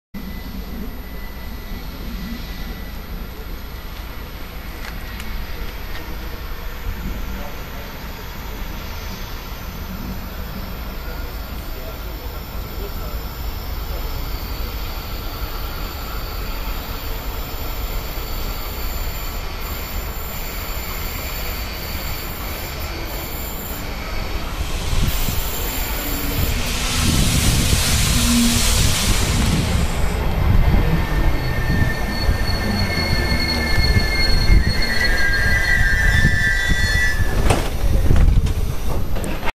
143 295 im Bahnhof Weißenfels
143 295 von Halle/Saale Hbf kommend bei der Einfahrt in den Bahnhof Weißenfels